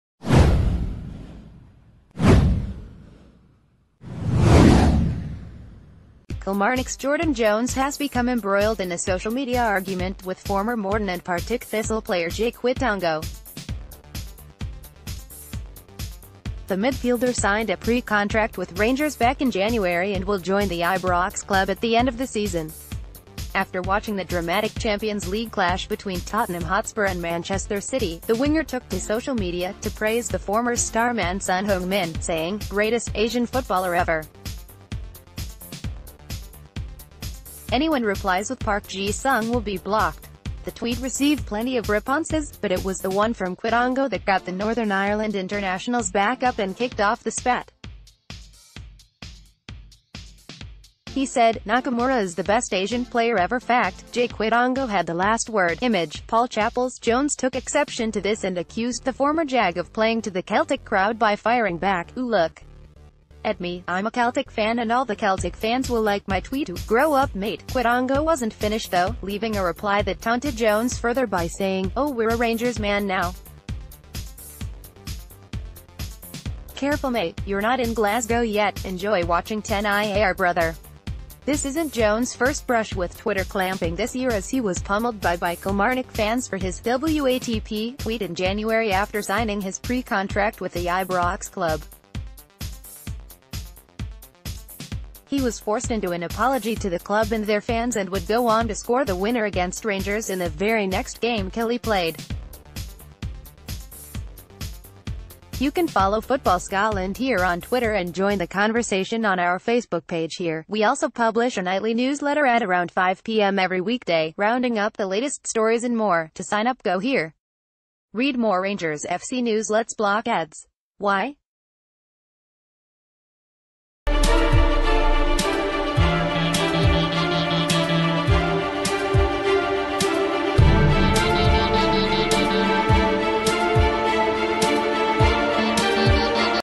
VIDEO: Incoming Rangers man Jordan Jones' Twitter beef with 'Celtic fan' Jai Quitongo
Kilmarnock's Jordan Jones has become embroiled in a social media argument  with former Morton and Partick Thistle player Jai Quitongo. The midfielder signed a pre-contract with Rangers back in January and will join the Ibrox club at the end of the season. After watching the dramatic Champion's League clash between Tottenham Hotspur and Manchester City, the winger took to social media to praise the former's star man Son Heung-Min, saying: "Greatest Asian footballer ever. Anyone reply’s with Park ...